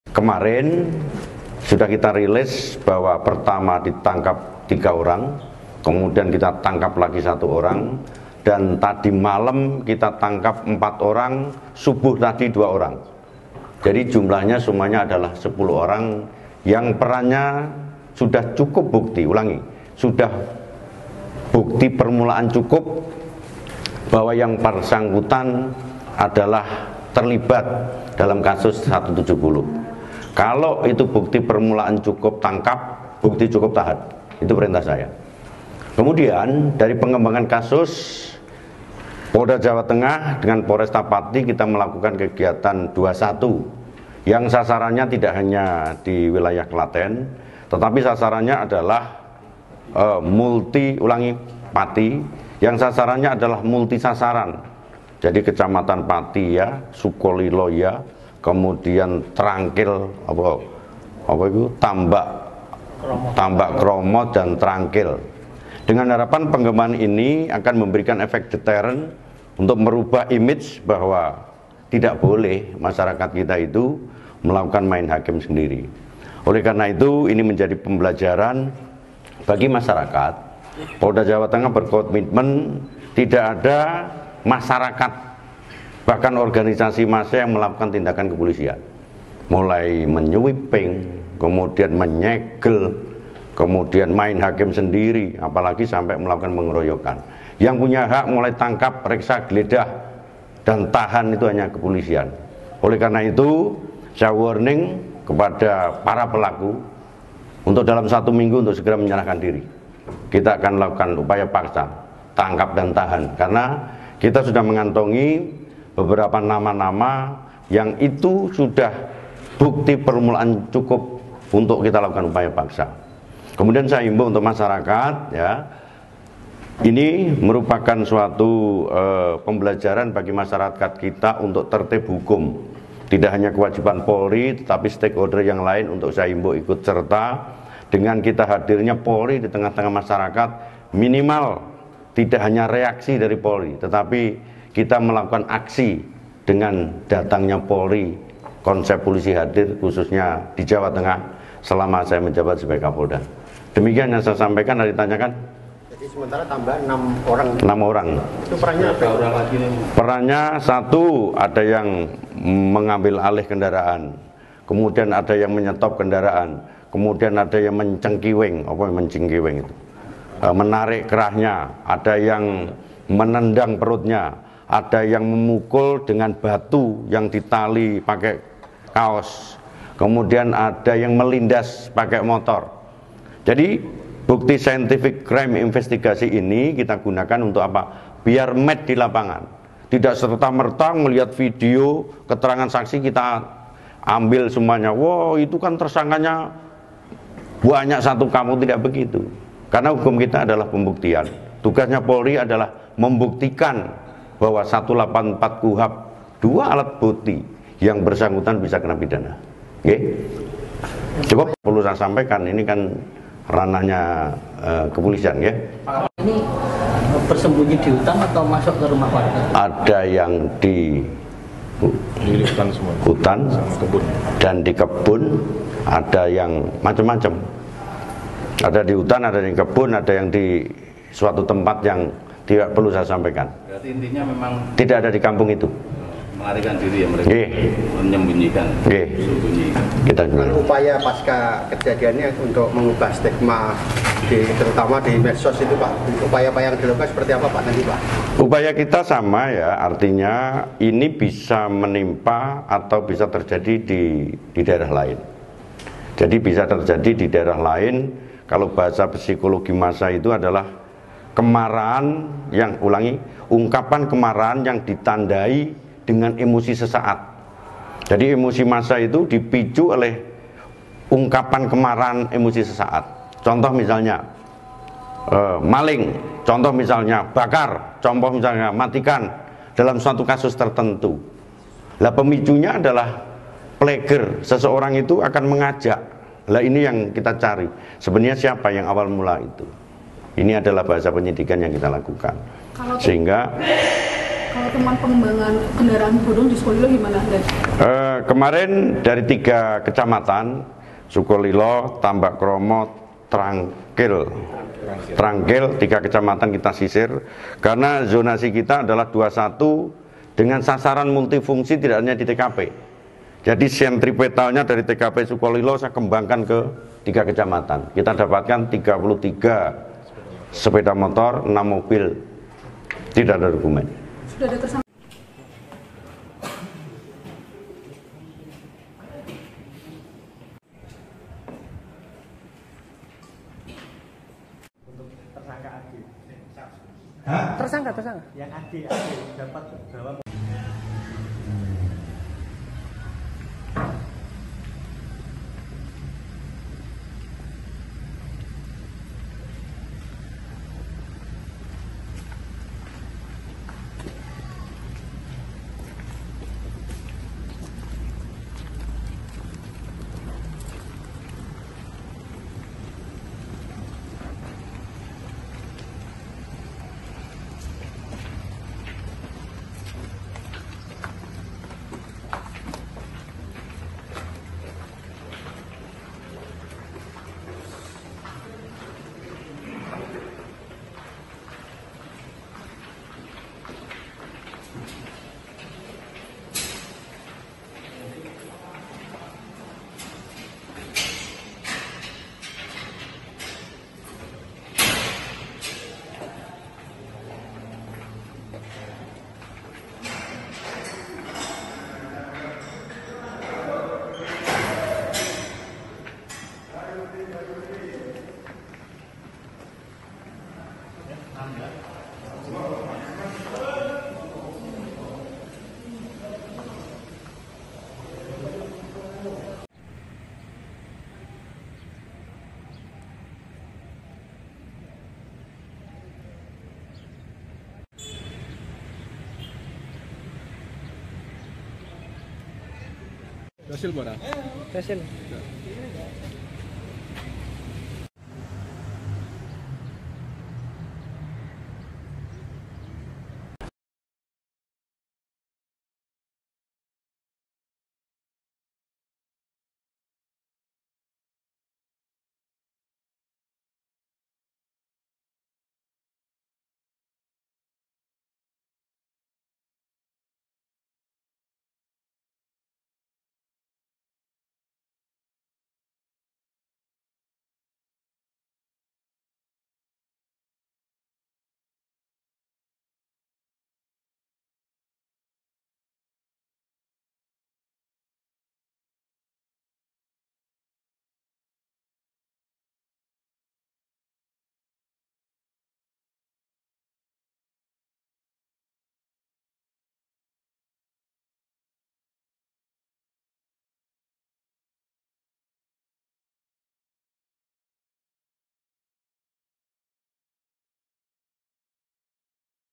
Total 10 Tersangka, Polisi Bekuk 6 Terduga Pelaku Baru Pengeroyokan Bos Rental di Pati
Polisi mengungkapkan perkembangan terkini dari kasus bos rental mobil asal Jakarta yang dikeroyok hingga tewas di Sukolilo, Kabupaten Pati, Jawa Tengah. Satu per satu pelaku yang sempat bersembunyi kini ditangkap.

Tim Gabungan Polres Pati dan Polda Jawa Tengah kembali meringkus 6 terduga pelaku baru. Keenam terduga pelaku tersebut terdiri dari satu orang warga Desa Sumbersoko dan lima orang warga Desa Tompegunung. Total sudah ada 10 orang yang diamankan polisi.

"Tadi malam kami tangkap 4 orang, subuh tadi 2 orang," ungkap Kapolda Jateng Irjen Ahmad Lutfi di Mapolda Jateng, Semarang, Sabtu (15/6/2024).

Kapolda juga mengungkapkan peran para tersangka masing-masing berbeda-beda. Di antaranya menyeret korban, memukul, dan menendang korban, menghantam batu ke korban dan melindas korban dengan sepeda motor.

"Ada yang pakai batu ditali ke kaus dipukulkan ke korban, ada yang ambil alih kendaraan, ada yang menendang perut," lanjut Irjen Pol Ahmad Lutfi.